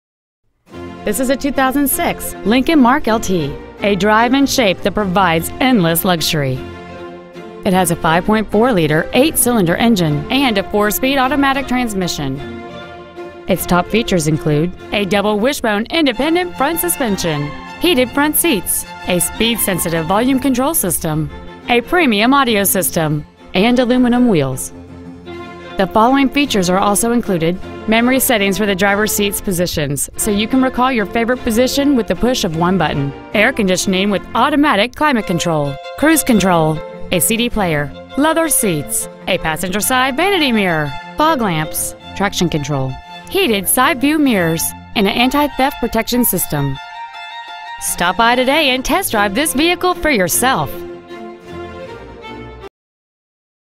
2006 Lincoln Mark LT Toccoa GA 30577
We are proud to present this 2006 Lincoln Mark LT .

 Year : 2006
 Make : Lincoln
 Model : Mark LT
 Engine : Gas V8 5.4L/330
 Trans . : Automatic
 Exterior : White
 Miles : 93,501
 Interior : Other
 MAYPOLE CHEVROLET, INC.
 2625 Hwy 17
 Toccoa , GA 30577
 Used 2006 Lincoln Mark LT Toccoa GA